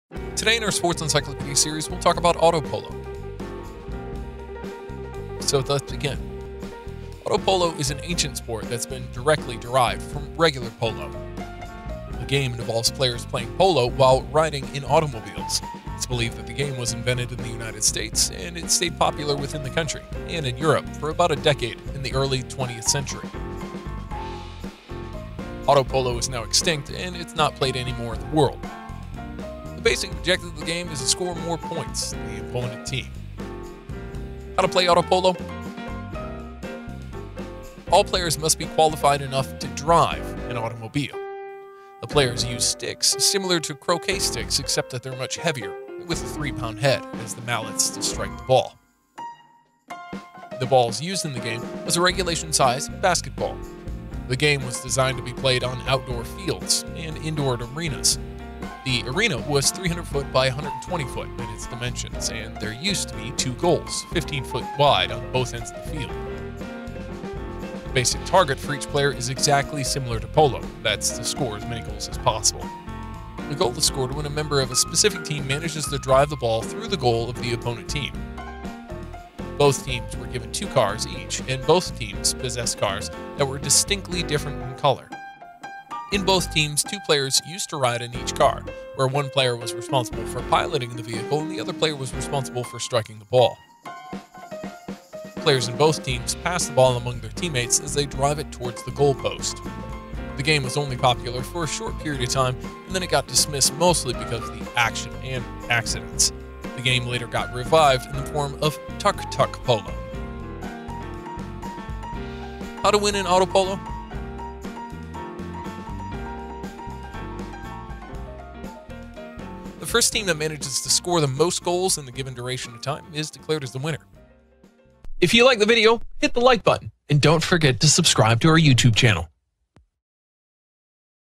How to Play Auto Polo? an ancient sport that involves players playing polo while riding automobiles.
Timestamps OR Chapters:
0:00 Introduction of Auto Polo
0:33 How to Play Auto Polo?
1:52 How to Win Auto Polo?

Today, in our sports encyclopedia series we will talk about Auto Polo.
Auto Polo is an ancient sport that has been directly derived from regular Polo.
The game involves players playing polo while riding automobiles.
It is believed that the game was invented in the United States and it stayed popular within the country and in Europe for about a decade in the early 20th century.
Auto Polo is now extinct and is not played anymore in the world.
The basic objective of the game is to score more than the opponent team.
All players must be qualified enough to drive an automobile.
The players used sticks similar to the croquet sticks except they had a much heavier 3 pound head, as the mallets to strike the ball.
The ball used in the game was a regulation size basketball.
The game was designed to be played on outdoor fields and indoor arenas.
The arena was 300 x 120ft in its dimensions and there used to be two goal posts, 15ft wide, on both ends of the field.
The basic target for each player is exactly similar to polo that is to score as many goals as possible.
A goal is scored when a member of a specific team manages to drive the ball through the goal of the opponent team.
Both teams were given two cars each and both the teams possessed cars that were distinctly different in colors.
In both teams, two players used to ride on each car, where one player was responsible for piloting the vehicle and the other player was responsible for striking the ball.
Players in both teams pass the ball among their teammates as they drive it towards the goal post.
The game was only popular for a very short period of time and then it got dismissed mostly because of the action and accidents.
The game later got revived in the form of Tuk-Tuk Polo.
The first team that manages to score the most goals in the given duration of time is declared as the winner.